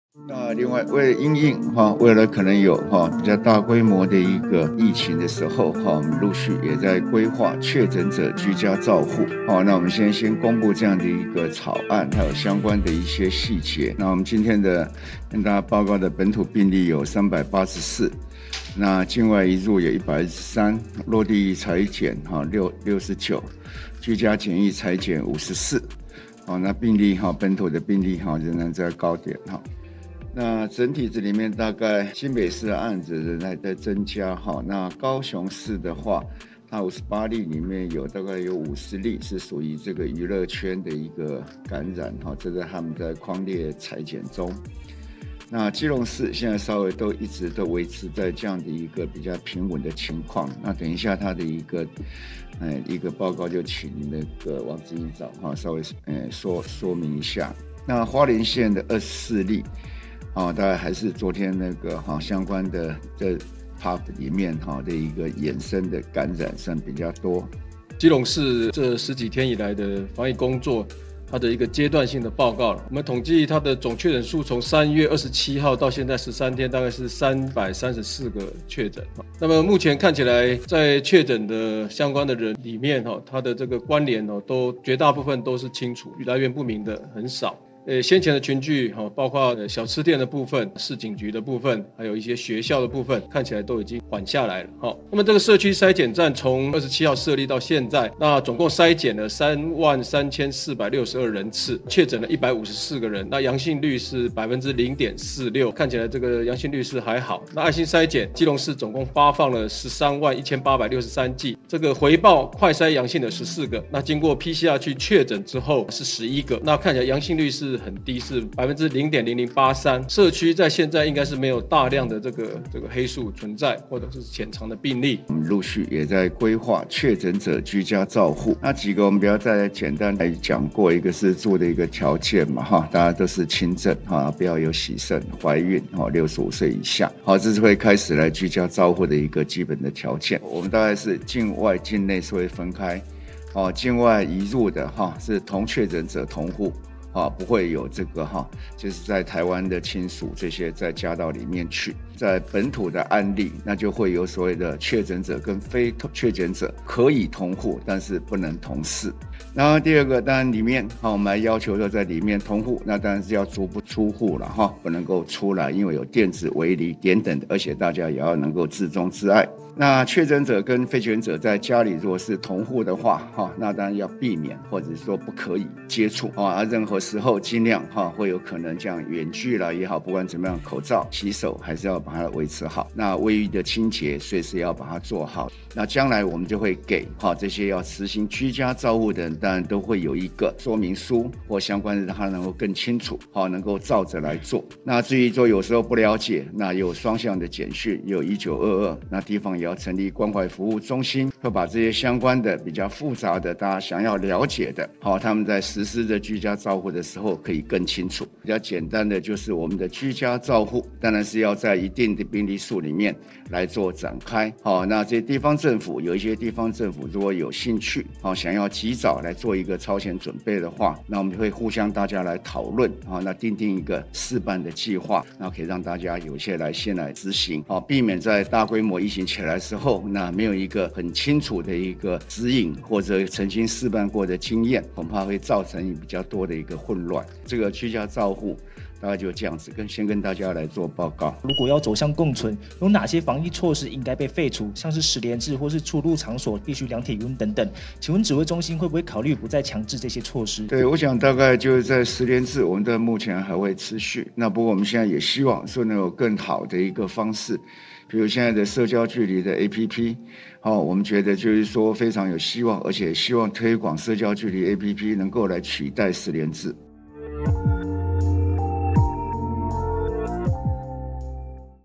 確診數再破500例！本土+384仍在高點「輕症在家」隔離指引公布
確診數再破500例！本土+384仍在高點新北最多 境外移入+123

中央流行疫情指揮中心指揮官陳時中公布，今日本土武漢肺炎（新型冠狀病毒病，COVID-19）病例新增384例，再度創下今年單日新高紀錄，本土病例仍在高點，境外移入新增123例，其中69例為落地採檢陽性。今日新增384例本土病例，為187例男性、197例女性，年齡介於未滿5歲至90多歲，發病日介於今年3